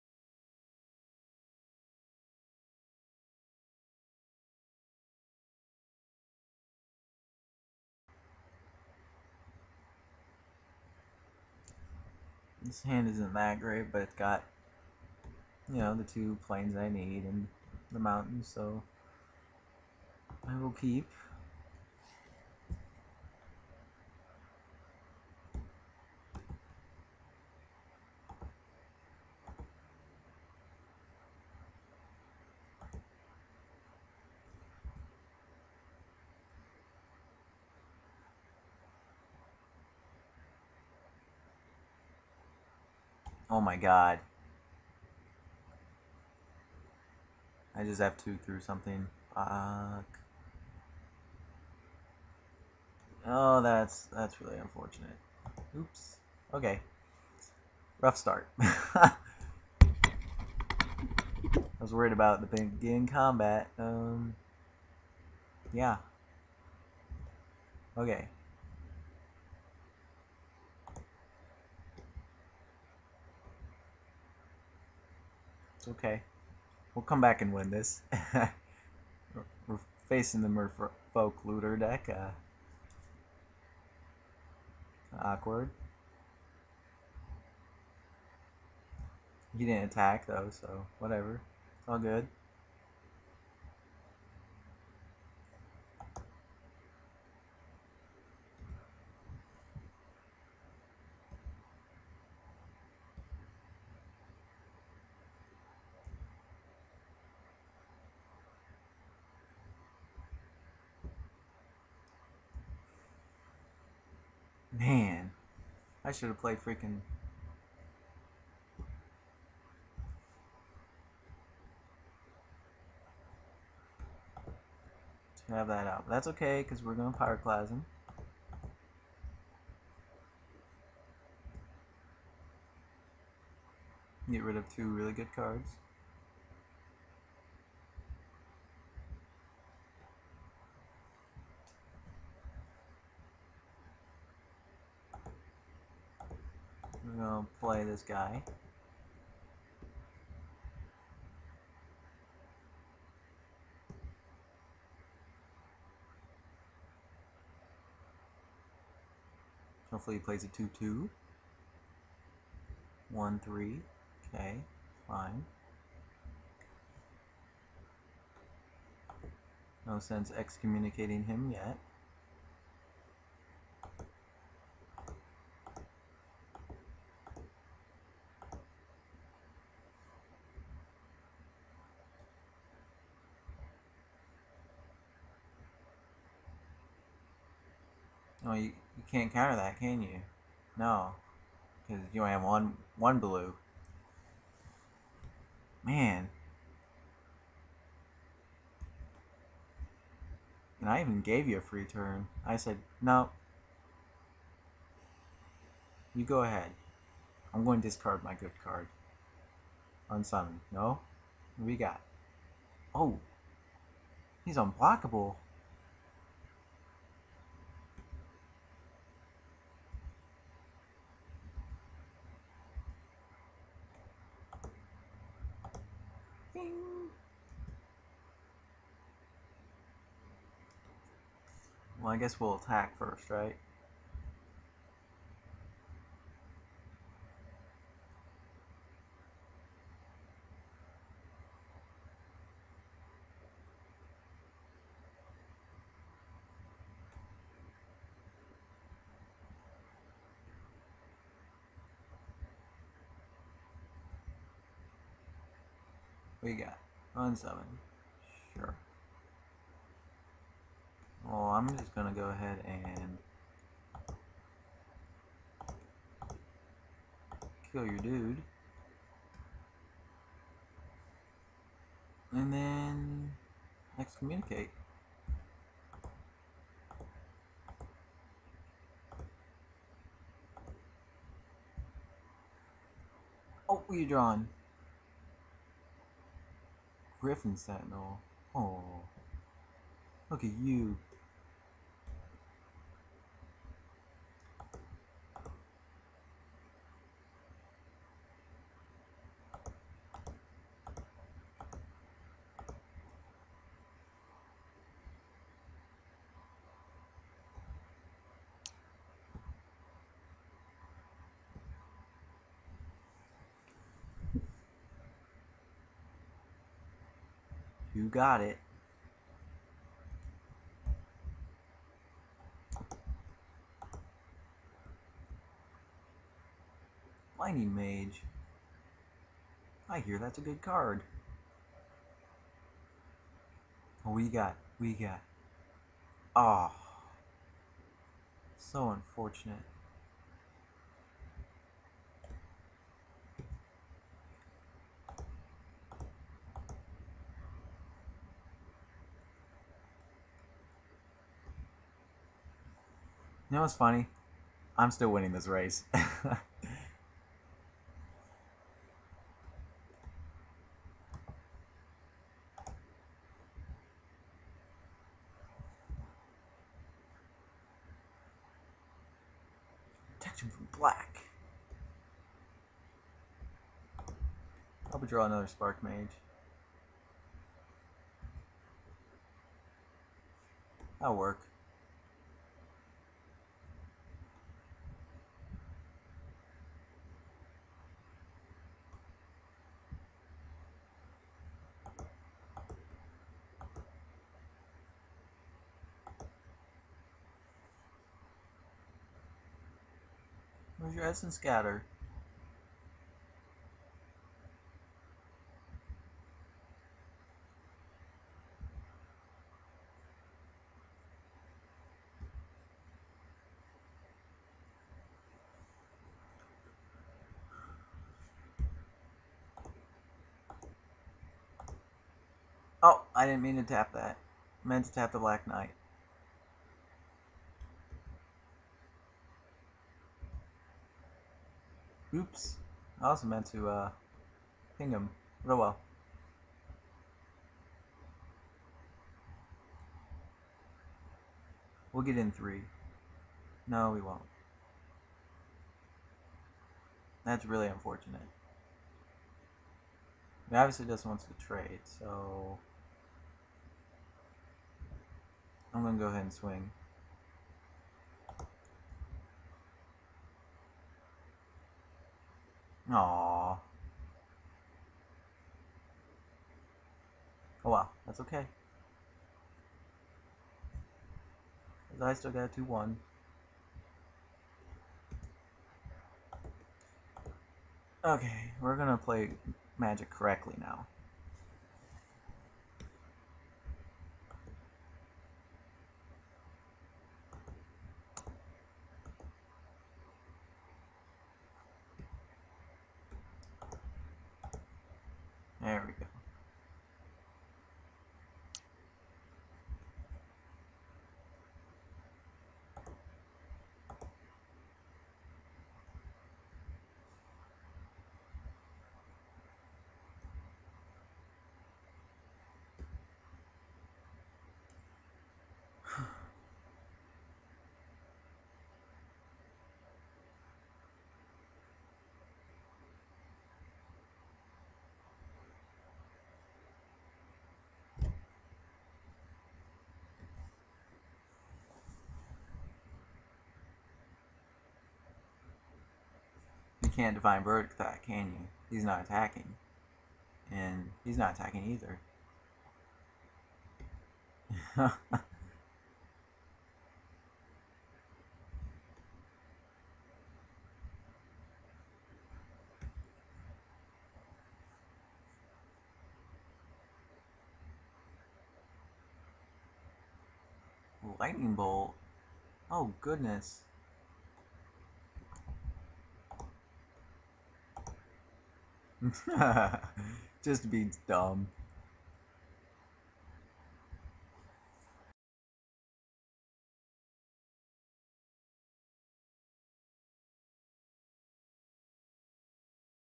[TCM]: M10 Draft 3 - Game 6 (Part 1 of 2)
TC sharing his thoughts of picks and plays during an M10 draft.
Please rate and leave comment on my play mistakes that you see.